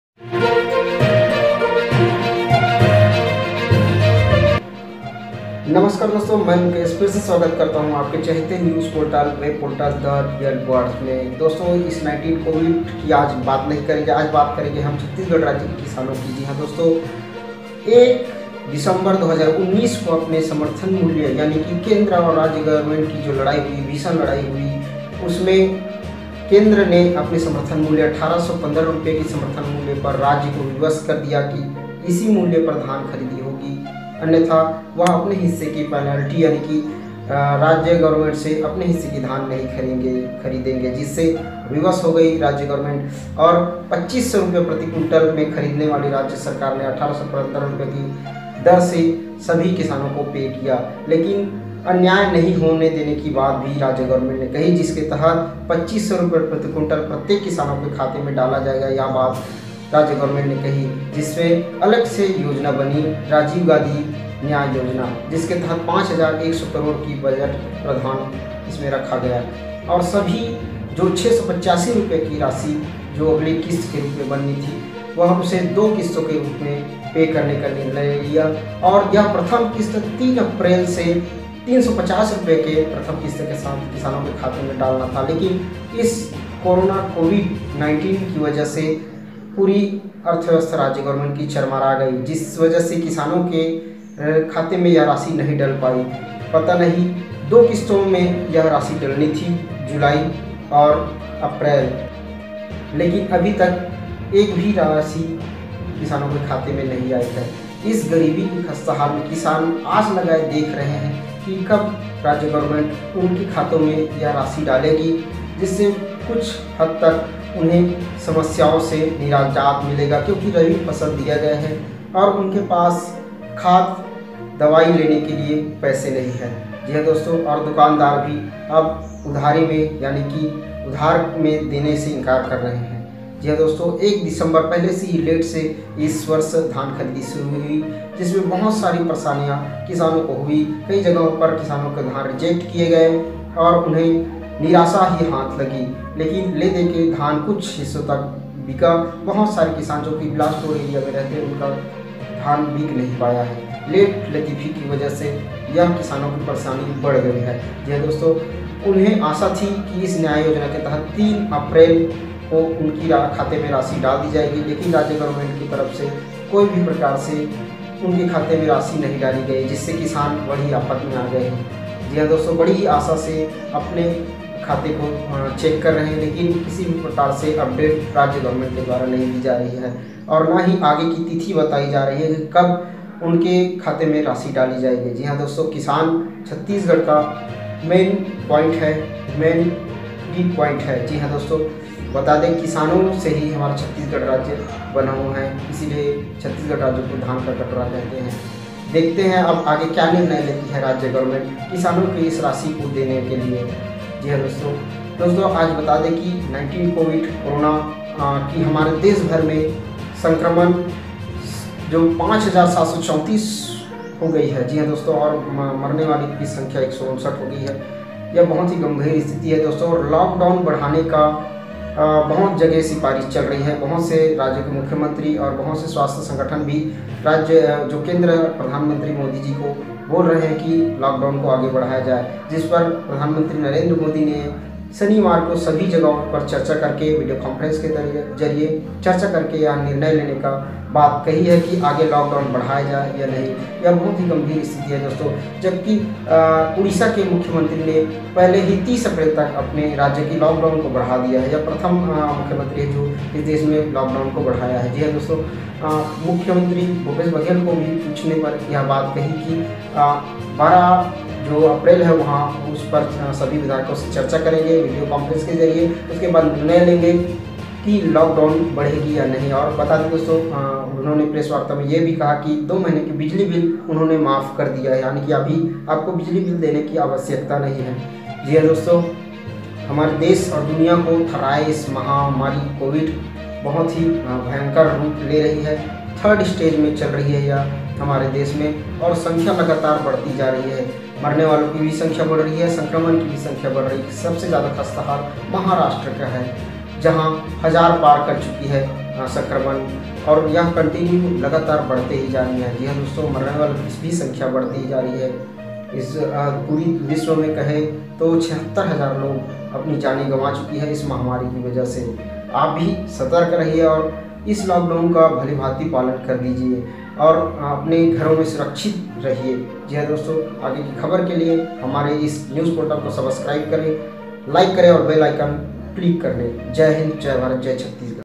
छत्तीसगढ़ समर्थन मूल्य की बचत राशि नहीं आई खाते में किसान चिंतित
दोस्त बता दे कि किसानों को 3 अप्रैल से समर्थन मूल्य की बाकी जो बचत  राशि ₹685 उसको आना था लेकिन नहीं डाला गया